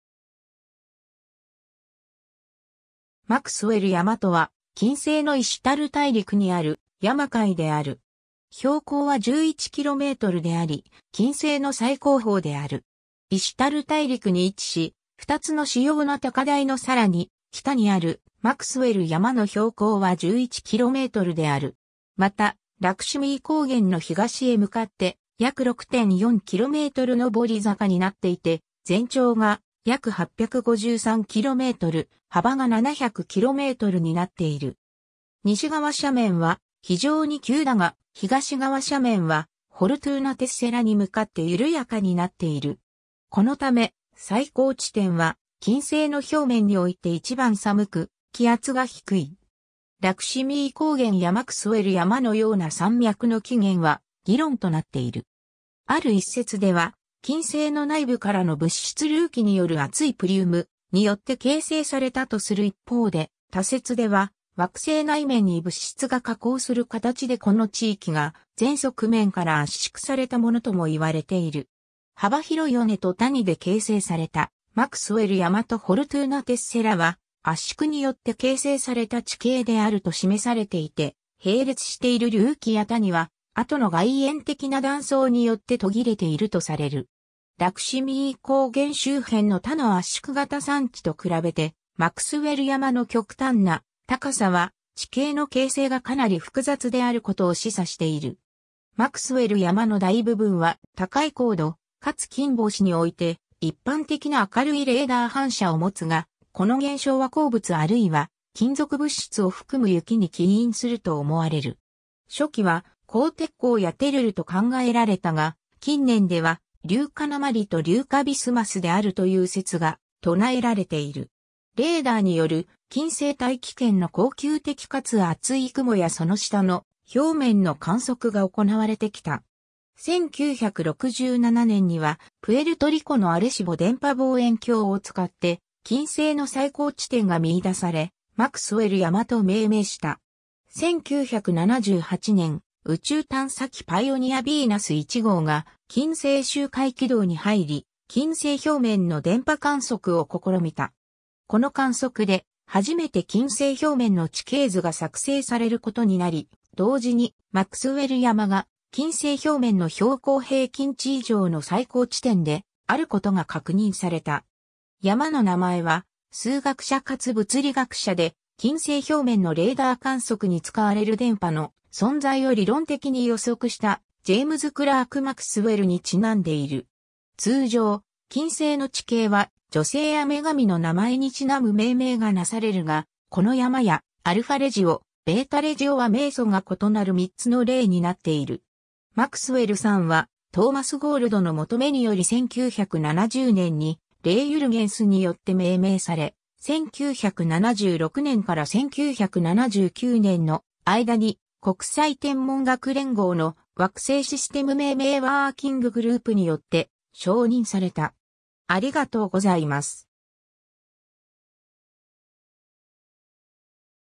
マクスウェル山
ジェームズ・クラーク・マクスウェル
マクスウェル山（英語: Maxwell Montes, [ˈmækswɛl ˈmɒntiːz]）とは、金星のイシュタル大陸にある山塊である。
標高は11kmであり、金星の最高峰である。
 イシュタル大陸に位置し、2つの主要な高台のさらに北にあるマクスウェル山の標高は11 km (6.8 mi) である。
またラクシュミー高原の東へ向かって約6.4 km (4 mi) 上り坂になっていて、全長が約853 km (530 mi)、幅が700 km (435 mi) になっている。
西側斜面は非常に急だが東側斜面はフォルトゥーナテッセラに向かって緩やかになっている。
このため最高地点は金星の表面において一番寒く（約380 °C または 716 °F）、気圧が低い（約45 bar or 44 atm）。
 ラクシュミー高原やマクスウェル山のような山脈の起源は議論となっている。
ある一説では金星の内部からの物質隆起による熱いプリュームによって形成されたとする一方で、他説では惑星内面に物質が下降する形でこの地域が全側面から圧縮（共に押しながら）されたものとも言われている。
幅広い尾根と谷で形成されたマクスウェル山とフォルトゥーナテッセラは圧縮によって形成された地形であると示されていて、並列している隆起や谷は後の外延的な断層によって途切れているとされる。
ラクシュミー高原周辺の他の圧縮型山地と比べてマクスウェル山の極端な高さは地形の形成がかなり複雑であることを示唆している。
 マクスウェル山の大部分は高い高度かつ金星において一般的な明るいレーダー反射を持つが、この現象は鉱物あるいは金属物質を含む雪に起因すると思われる。
初期は黄鉄鉱やテルルと考えられたが、近年では硫化鉛と硫化ビスマスであるという説が唱えられている。
 レーダーによる、金星大気圏の恒久的かつ厚い雲やその下の表面の観測が行われてきた。
1967年には、プエルトリコのアレシボ電波望遠鏡を使って金星の最高地点が見いだされ、マクスウェル山と命名した。
 1978年、宇宙探査機パイオニア・ヴィーナス1号が金星周回軌道に入り金星表面の電波観測を試みた。
この観測で初めて金星表面の地形図が作成されることになり、同時にマクスウェル山が金星表面の標高平均値以上の最高地点であることが確認された。
 山の名前は、数学者かつ物理学者で、金星表面のレーダー観測に使われる電波の存在を理論的に予測した、ジェームズ・クラーク・マクスウェルに因んでいる。
 通常、金星の地形は、女性や女神の名前に因む命名がなされるが、この山やアルファレジオ、ベータレジオは名祖が異なる3つの例になっている。
 マクスウェル山は、トーマス・ゴールドの求めにより1970年にレイ・ユルゲンスによって命名され、1976年から1979年の間に国際天文学連合の惑星システム命名ワーキンググループ (IAU/WGPSN) によって承認された。